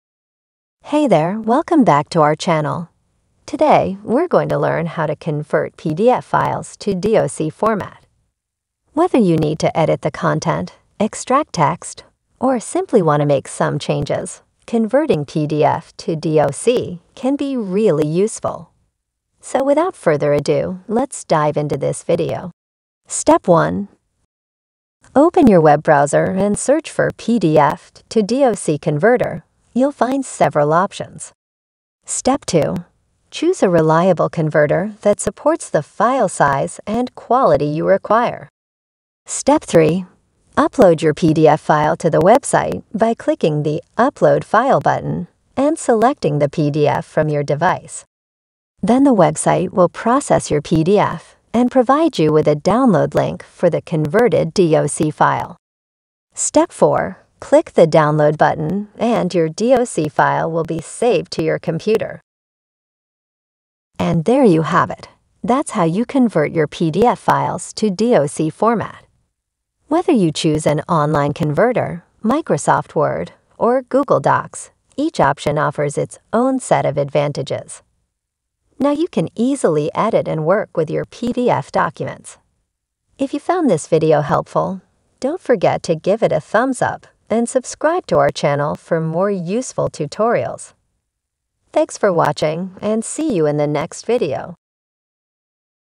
How To Convert PDF To DOC/WORD (PDF TO DOC CONVERTER)
How to Convert PDF to DOC/Word (PDF to DOC Converter) 📄🔄📝 [Step-By-Step Guide]

Need to edit or extract text from a PDF document in Microsoft Word? Converting PDF to DOC (Word) format is the perfect solution, allowing you to easily make changes and work with the content. In this step-by-step tutorial, we'll show you how to convert PDF files to DOC/Word using a reliable PDF-to-DOC converter.

🎥 In this video, you'll learn:

1️⃣ How to find and choose a trustworthy PDF to DOC converter.
2️⃣ A detailed guide to uploading your PDF file to the converter platform.
3️⃣ Tips on customizing the conversion settings, such as retaining formatting and images.
4️⃣ How to initiate the PDF to DOC conversion process and download the converted file.
5️⃣ Bonus: Exploring additional features of the PDF to DOC converter for advanced functionality.

Converting PDF to DOC enables you to edit, format, and collaborate with PDF content seamlessly in Microsoft Word.

If my videos helped you save money, then please consider sending me a tip instead!